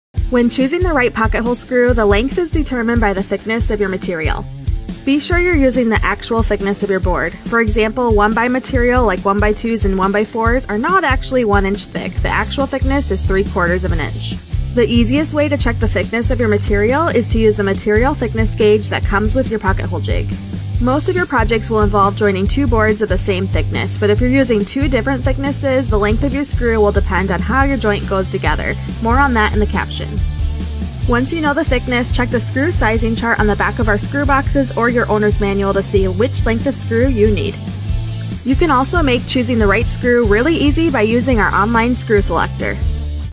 Screw Selection, Part 2
✨Screw Selection, Part 2: How thick is the wood you’re using?

And if you want a shortcut to finding the best screw for your project, check out our free online Screw Selector